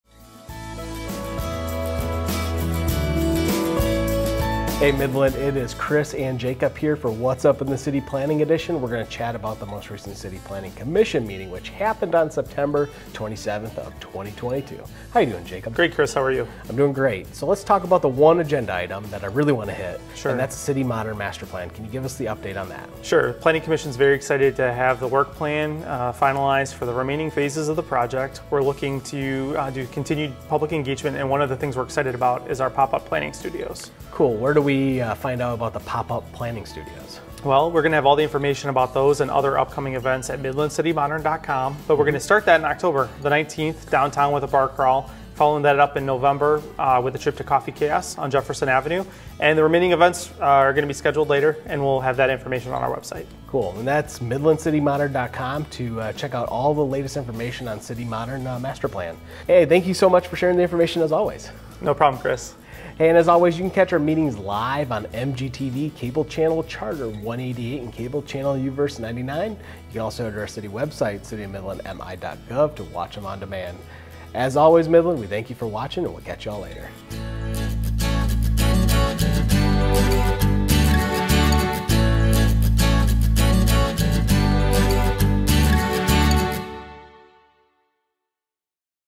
City of Midland | What's Up in the City! Planning: CIty Modern Pop-up Studios
At the September 27, 2022 City Planning Commission meeting, staff and the Commission finalized the schedule for completing the City Modern Master Plan update - which includes a series of pop-up studios at "third places" throughout the community. The first stop: A Downtown "bar crawl" on October 19!